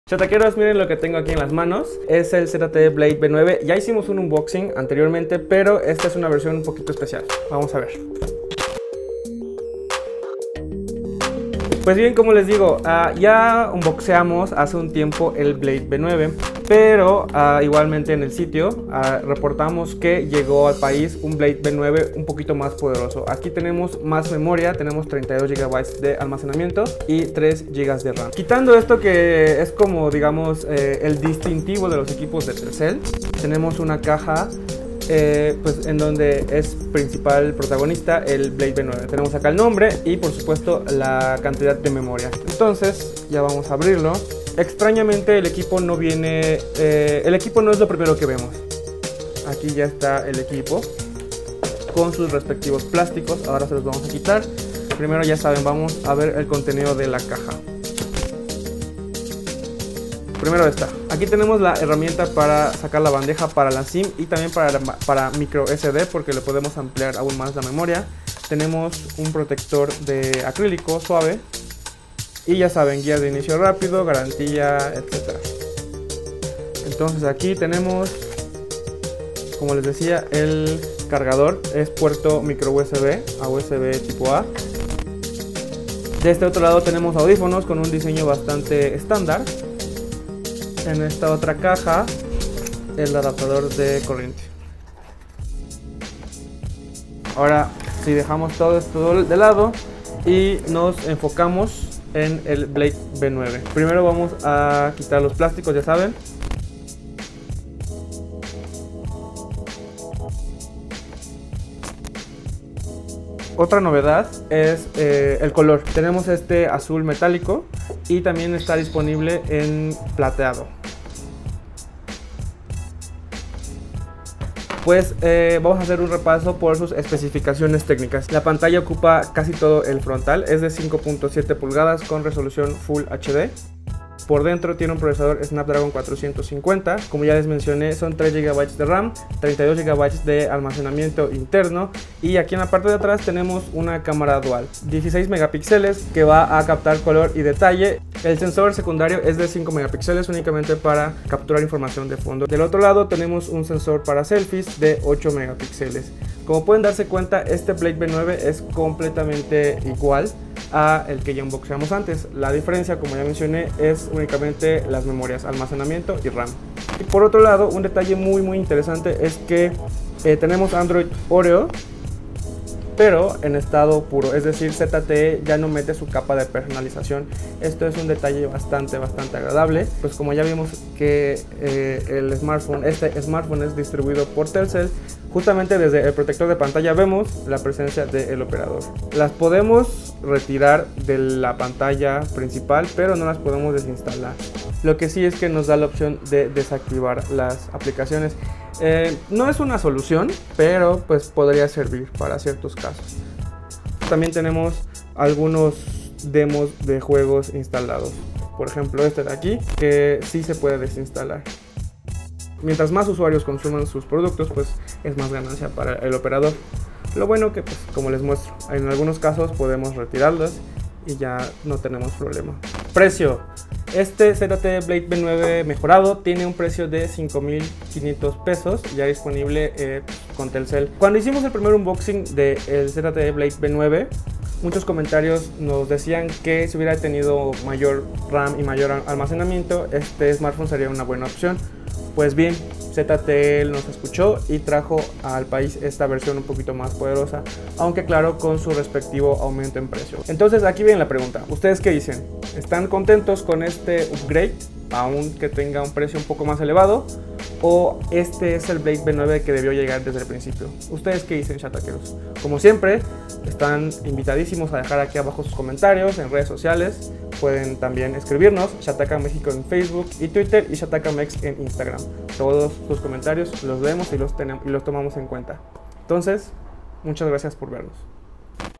ZTE Blade V9 (32GB), UNBOXING: más RAM, más colores y "Android Puro"
Parece que ZTE nos escuchó y lanzó una versión de su BladeV9 con 3 GB de RAM y 32 de almacenamiento interno. ¿Qué opinan?